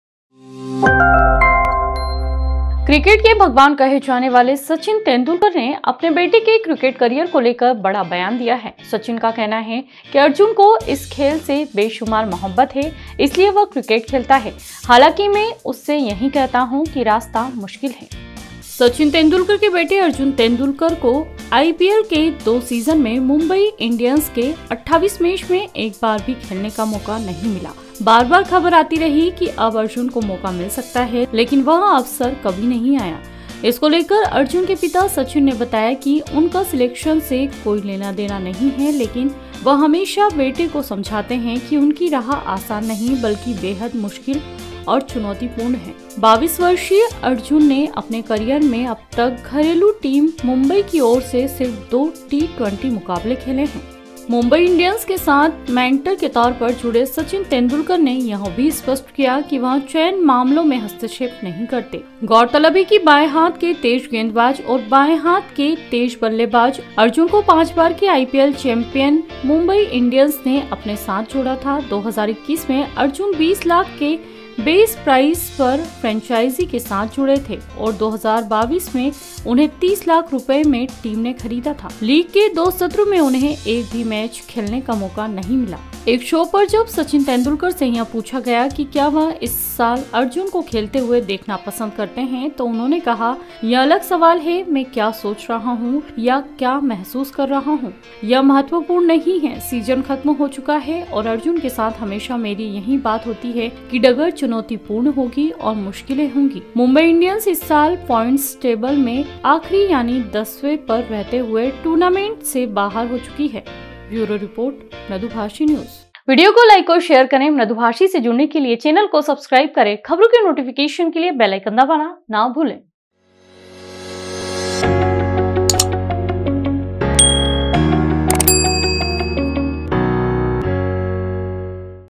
Arjun को IPL में नहीं मिला मौका, तो Sachin Tendulkar ने कही यह बड़ी बात | Mumbai Indians | IPL 2022
क्रिकेट के भगवान कहे जाने वाले सचिन तेंदुलकर ने अपने बेटे के क्रिकेट करियर को लेकर बड़ा बयान दिया है। सचिन का कहना है कि अर्जुन को इस खेल से बेशुमार मोहब्बत है, इसलिए वह क्रिकेट खेलता है। हालांकि, मैं उसे यही कहता हूं कि रास्ता मुश्किल है।